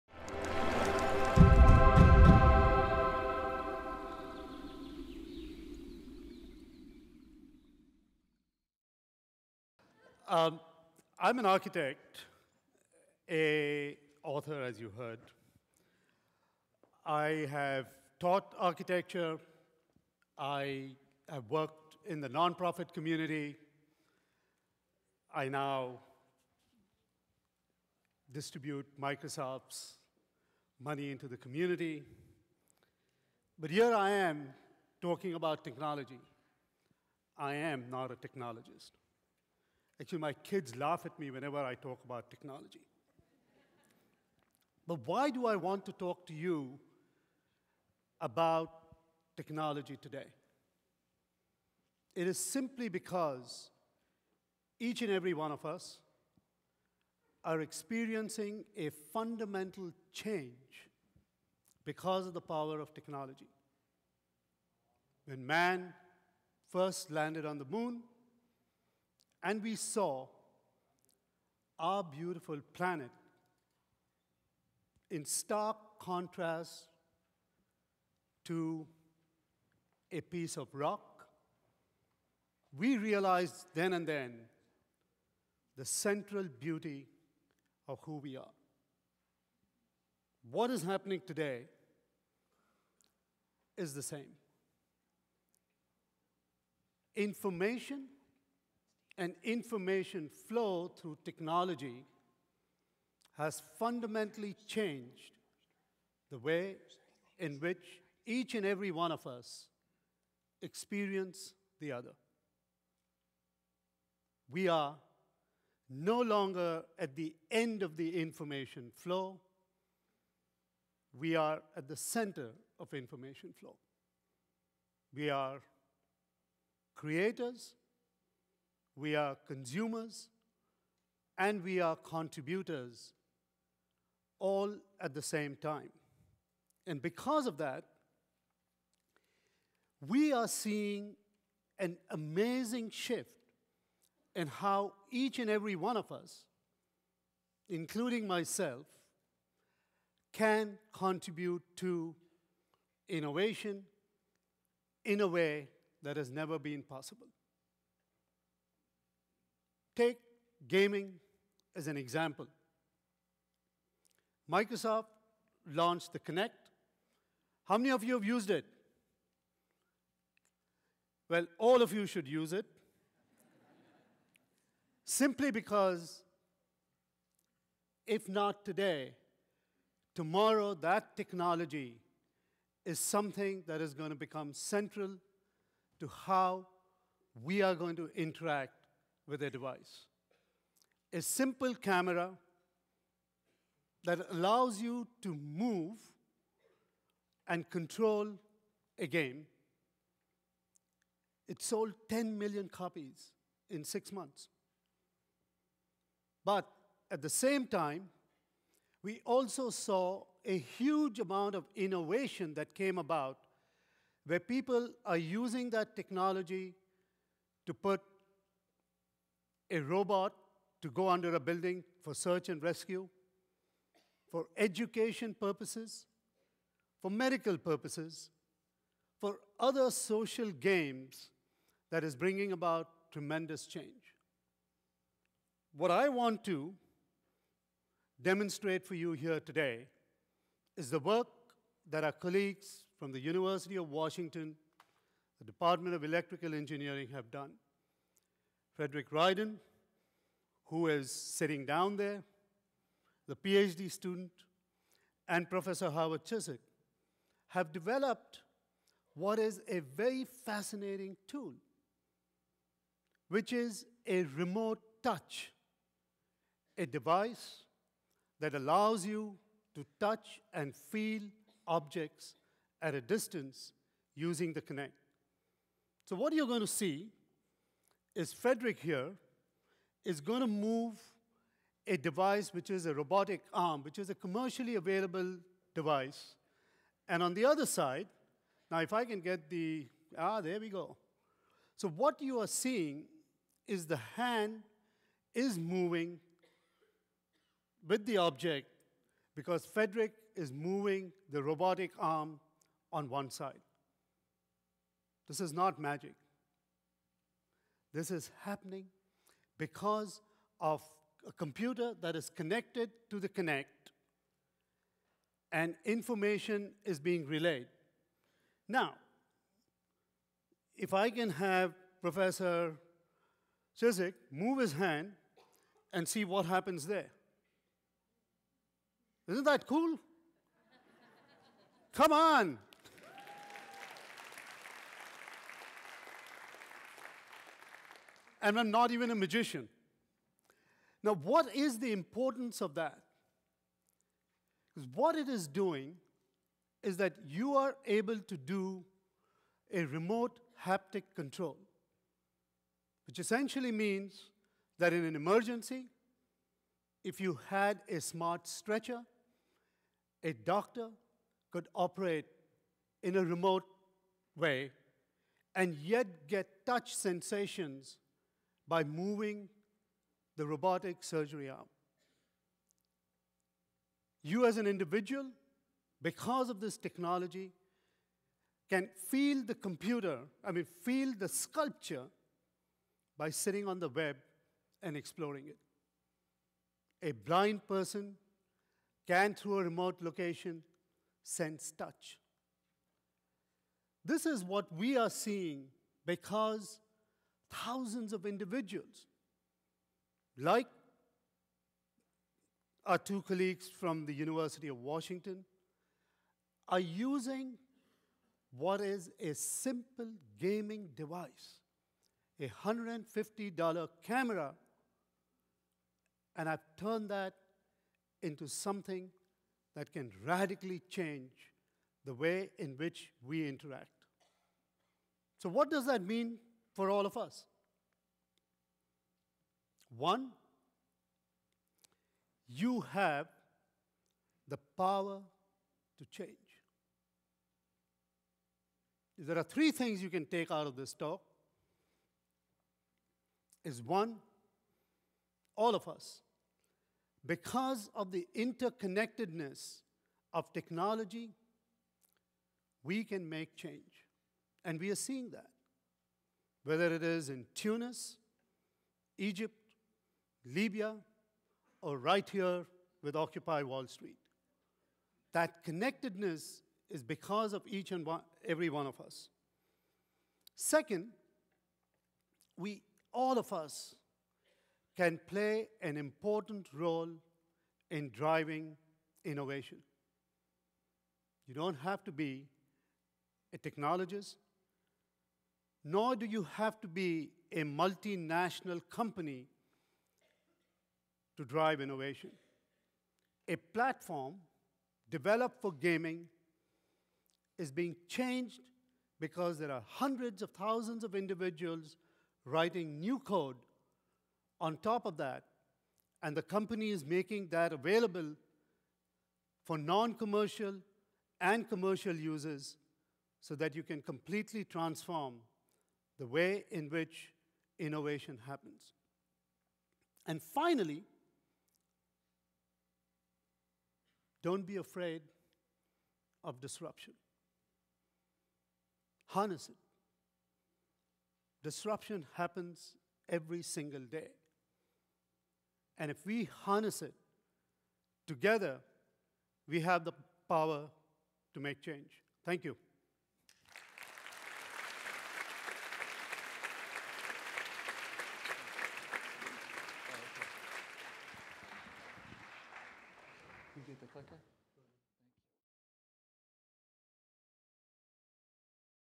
TEDxRainier - Akhtar Badshah - Harnessing Disruptive Technologies
Akhtar Badshah shares how technology is being used by community innovators to meet crucial needs.